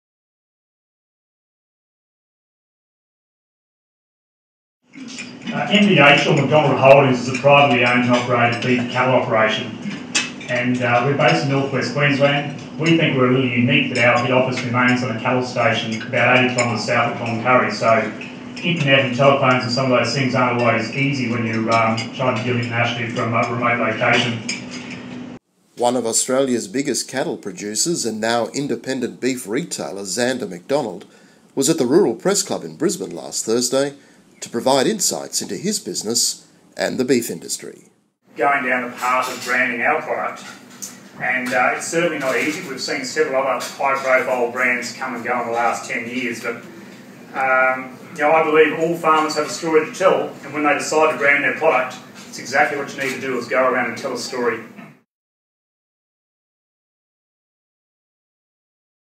McDonald tells producers to share their story
Zanda McDonald, general manager of MDH, provided plenty of food for thought at the Rural Press Club lunch on November 8. The cattle king turned super butcher encouraged producers to share their story in order to more effectively promote their product.